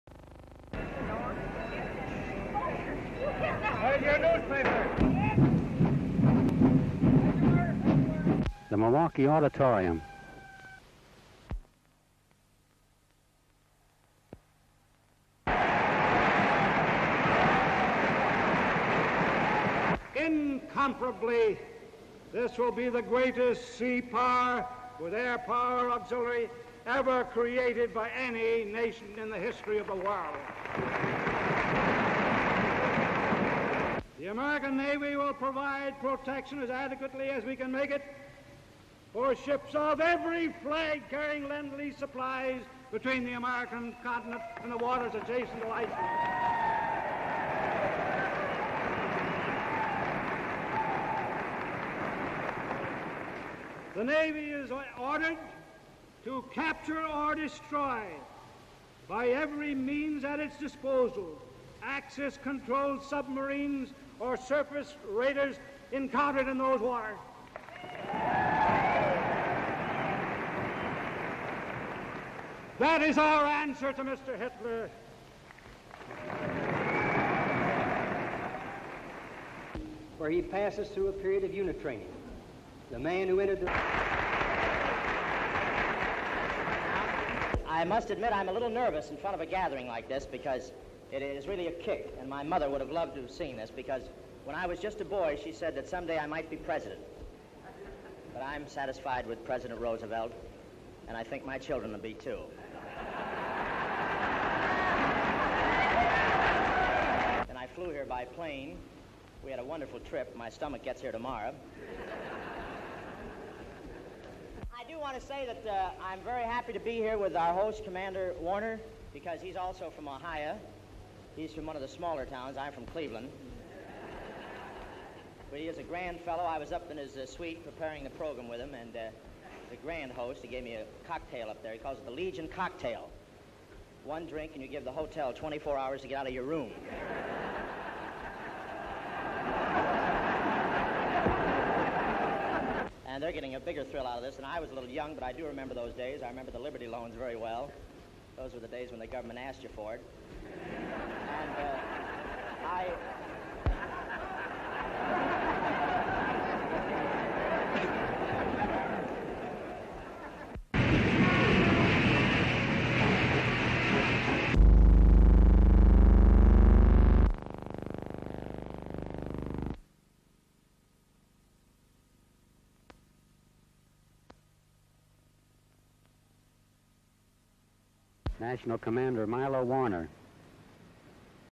U.S Navy Secretary Frank Knox and entertainer Bob Hope addresses American Legion of vetera...(1941)
At American Convention , Great War Veterans Urge Repeal of Neutrality Act and the Endorsement of the two-ocean Navy Programme.

Full Description:

Gen. view of train and crowd. Semi view pan of sign "War Extra. Germany wants Russia. Japan wants China. China wants Laundry."
Long view Legionnaires marching carrying 3 flags 1 is Stars and Stripes.
Gen. view Convention Hall. Interior shot of convention. Semi view Legionnaires applauding. Semi view Secretary Knox talking. Long view Legionnaires applaud. Semi view Knox talking.

Sound Effects. Semi view Knox speaking: "Incomparably, this will be the greatest sea power with air power auxiliary ever created by any nation in the history of the world. The American Navy will provide protection as adequate as we can make it for the ships of every flag carrying lend-air supplies between the American continent and the waters adjacent to Iceland". (Applause) The Navy is ordered to capture or destroy by every means at their disposal, Axis-controlled submarines or surface raiders encountered in those waters." (Applause) "That is our answer to Mr. Hitler." (Applause).

Gen. view auditorium. Long view Bob Hope gets up from table and walks to mike. Semi close up Bop Hope talking. Semi close up Hope talking. Long view Hope talking. Semi view Hope talking.

Sound Effects. Semi close up Bob Hope speaking "I must admit I am a little nervous in front of a gathering like this because it is really a kick and my mother would love to have seen it because when I was just a boy she said that some day I might be President but I am satisfied with President Roosevelt and I think my children will be too. (Applause).

Semi close up Bob Hope "I flew here by 'plane. We had a wonderful trip...my stomach gets here to-morrow." (Laughter).

Semi view Bob Hope talking "I do want to say that I am very happy to be here with out host Commander Waters because he is also from Ohio, he is from one of the smaller towns, I am from Cleveland but he is a grand fellow. I was up in his suite preparing a programme with him and he is a grand host. He gave me a cocktail up there - he calls it the Legion cocktail - One drink and you give the hotel 24 hours to get out of your room (Laughter).

Long view Bob Hope talking "They are getting a bigger thrill out of this... When I was a little younger - I do remember those days. I remember the Liberty Loans very well. Those were the days when the Government asked you for it." (Laughter).

Good Scenes. Gen. view train around curve in open town. Semi view people on boat. Semi view people off boat. Gen. view people off United Airlines 'plane. Semi view people off plane. Semi pose view Hollywood Delegation, including Hedda Hopper. Semi pose view of Commander (Milo)  Warner.

United States of America; Personalities - Politicians; Personalities - Performers; Ex-servicemen; Colonel Frank Knox (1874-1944), Bob Hope (b.1903)
Background: At American Convention , Great War Veterans Urge Repeal of Neutrality Act and the Endorsement of the two-ocean Navy Programme. Navy Secretary Co. Knox speaks forcibly on the Subject.